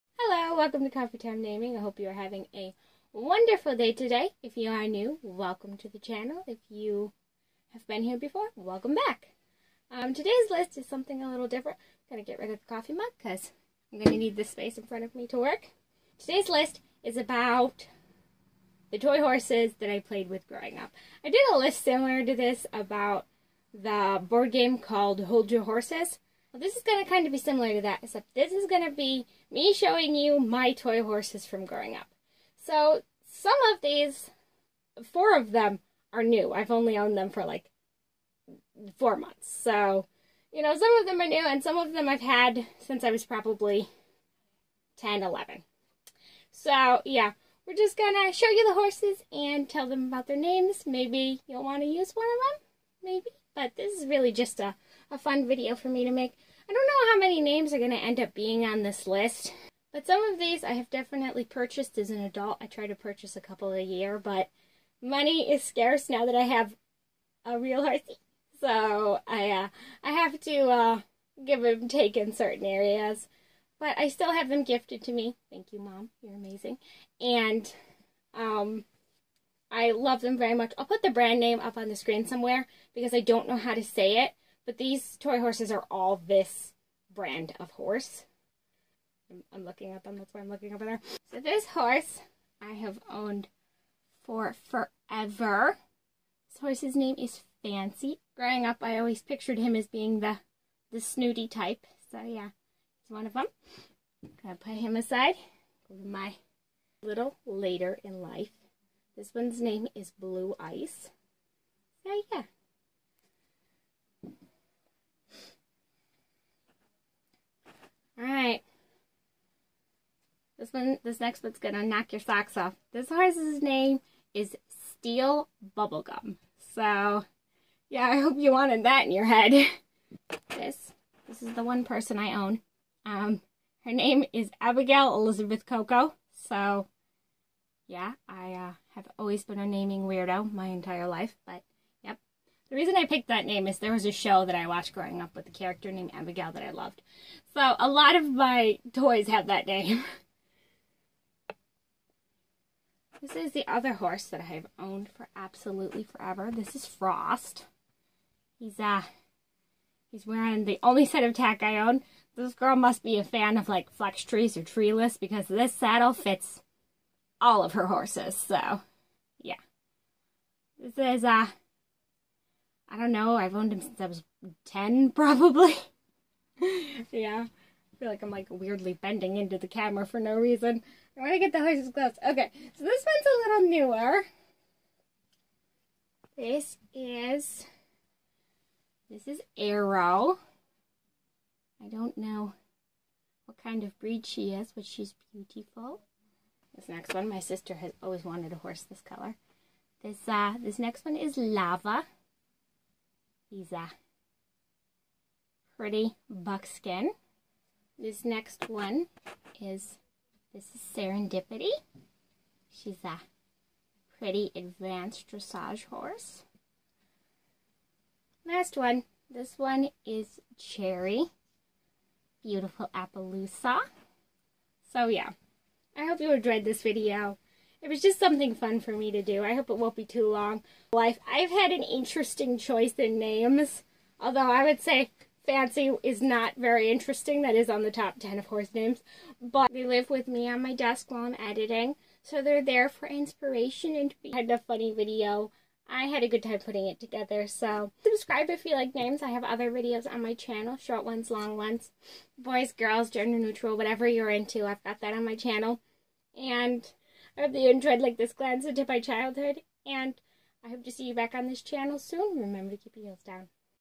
Meet And Learn The Names Of My Childhood Schleich Toy Horses
A walk down memory lane learn about my ten-year-old self-name choices